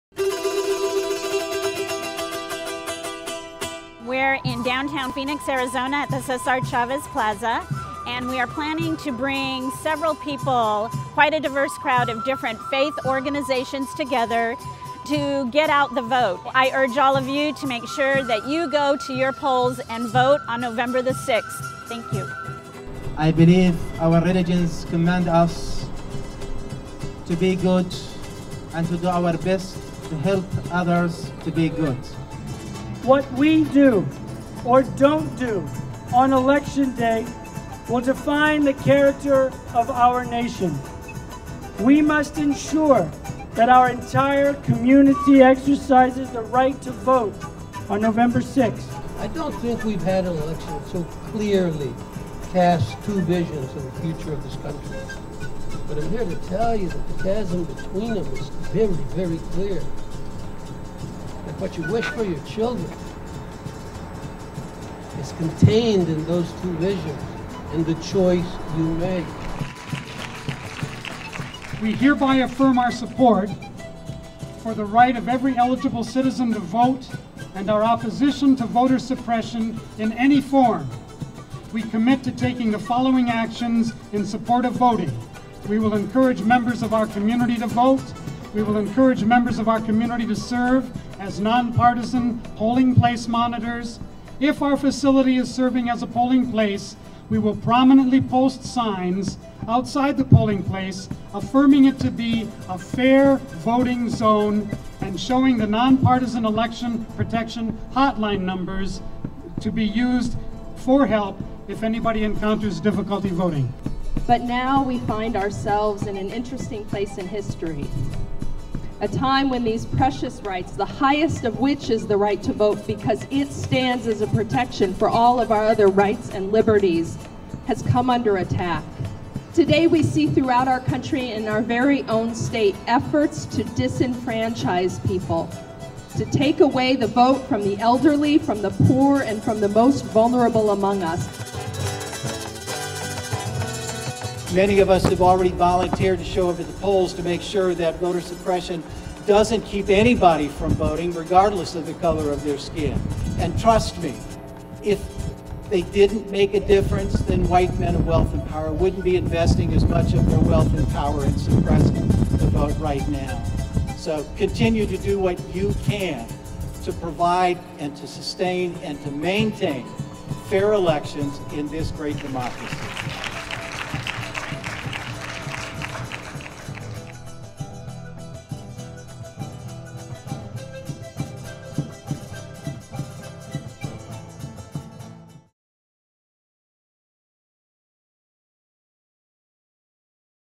You Count! Arizona Interfaith Coalition
The Arizona Interfaith Coalition put together You Count! a get out the event where people of all faiths gathered to support voter rights. Faith leaders from across Arizona pledged to mobilize their members to vote, encourage their members to volunteer at polling sites and if their church, mosque or temple is serving as a polling site, to ensure that it is a polling safe zone.

The Arizona Interfaith movement's mission is to build bridges of understanding, respect, and support among diverse people of faith through education, dialogue, service, and the implementation of the Golden Rule.